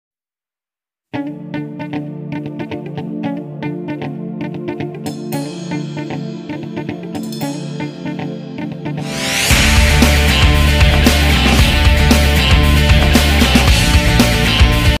Camera Alexa mini lf shooting Al wafra eria 2022 drama
alexa
mini Lf
camera alexa
advertising in Al wafra eria all farms there nice weather for shooting
car shooting